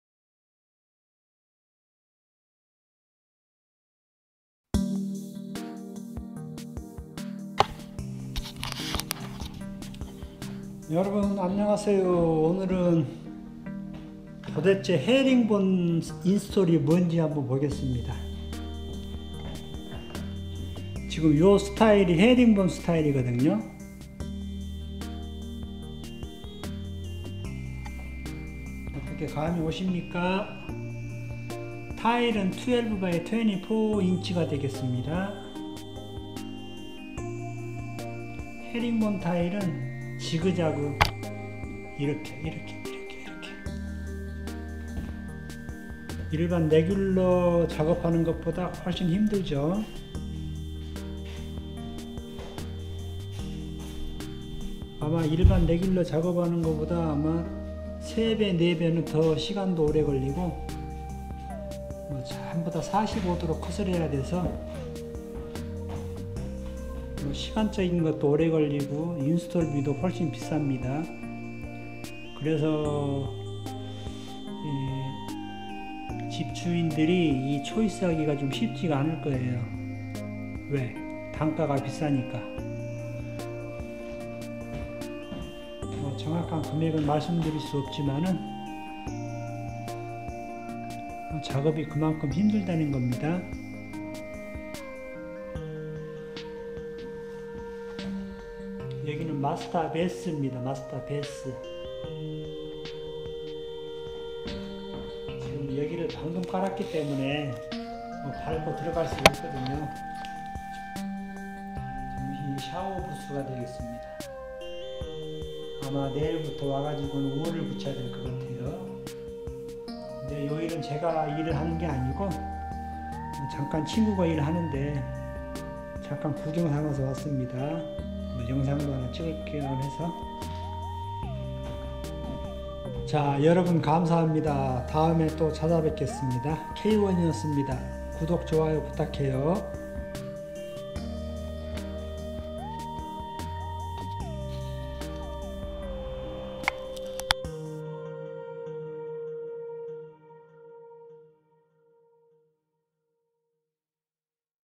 타일 해링본 스타일이 무엇???? Herringbone Style Install k-one
도대체 해링본 스타일이 무엇인가?
일반 작업보다 3~4배는 힘든 작업입니다